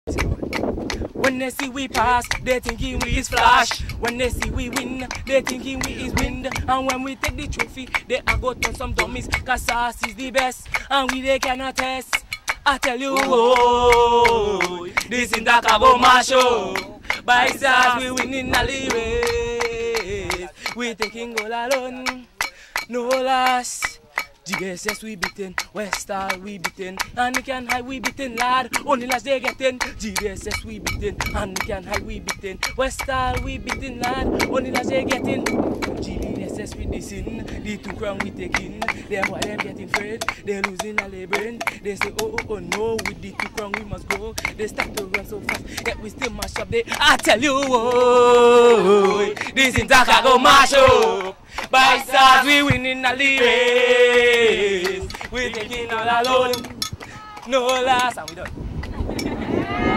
SASS chant| Intercol 2015
St. Andrews Anglican Secondary School, chant build-up for Intercol 2015.